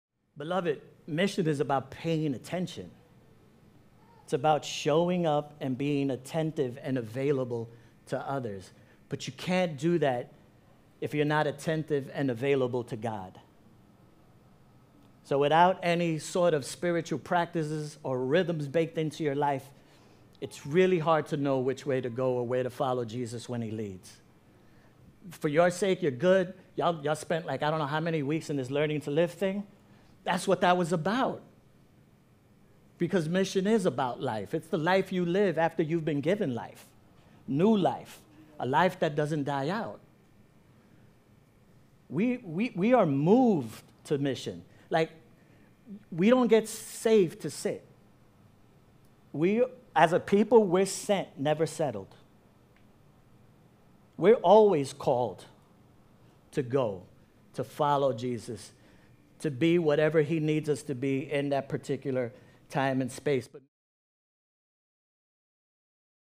Paying Attention
"Mission is about paying attention. It is about showing up and being attentive and available to others."

Excerpt from "Strengthening the Core: Mission" | May 5, 2024